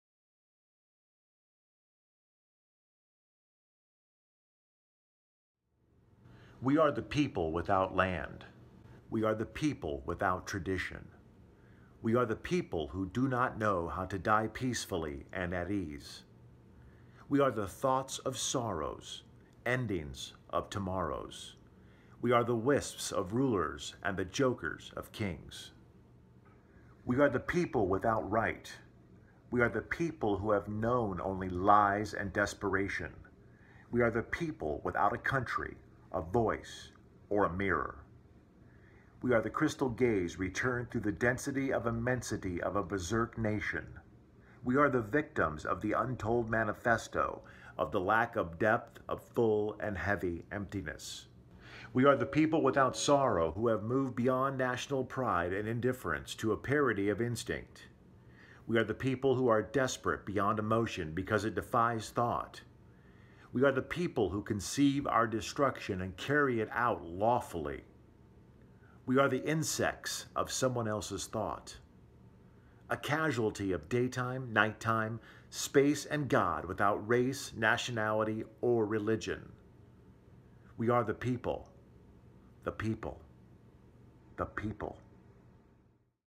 We Are The People - Lou Reed Poem (Poetry Reading)
Just about everything in this video is not good, right down to my shirt. Thankfully, the poem stands alone. and God created B&W so I could hide the shirt colors during the reading.

This is an obvious departure from my regular videos, and I promise not to saturate my channel with too many of these. But I wanted to try something different. and now I know it needs some work.

The camera is too close to my face so you can see me reading the poem. I couldn't do it all in one take because there are a couple of sentences that are so grammatically poor, I kept stumbling through them when reading. My head moves all over the place. This is essentially what happens when I'm doing voiceover work. I guess it's the way I put more emotion in my voice. Which, by the way, there's a more chill version of me reading this poem but I nixed it for that reason - it came across as boring. (imo)

Anyhoodles (Brockmire)...

A year or so ago I read the Lou Reed book of poems, cleverly titled: "Do Angels Need Haircuts?"

I just happened to come across this one again, and decided it was appropriate for the year 2020.

I also just realized that there's a version of Iggy Pop reading this poem online. I am not going to watch/listen to THAT right now because if I do I am likely to become embarrassed and never post this. Honestly, though, I just wanted to share Lou Reed's poem with you. So.

We Are The People
A poem by Lou Reed

We are the people without land.
We are the people without tradition.
We are the people who do not know how to die peacefully and at ease.
We are the thoughts of sorrows.
Endings of tomorrows.
We are the wisps of rulers and the jokers of kings.
We are the people without right.
We are the people who have known only lies and desperation.
We are the people without a country, a voice or a mirror.
We are the crystal gaze returned through the density and immensity of a berserk nation.
We are the victims of the untold manifesto of the lack of depth of full and heavy emptiness.
We are the people without sorrow who have moved beyond national pride and indifference to a parody of instinct.
We are the people who are desperate beyond emotion because it defies thought.
We are the people who conceive our destruction and carry it out lawfully.
We are the insects of someone else’s thought.
A casualty of daytime, nighttime, space and god without race, nationality or religion.
The people.
The people.